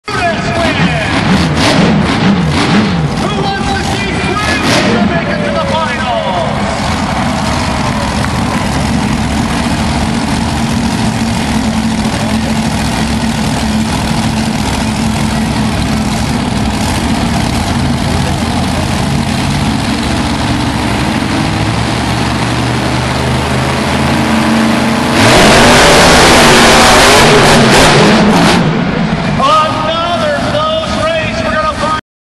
Monster Jam: Pittsburgh, PA-Racing 6/7
The second semi-final match up had teammates from the previous race go head-to-head.  Chris Bergeron in Brutus goes against Pablo Huffaker in the Grave Digger.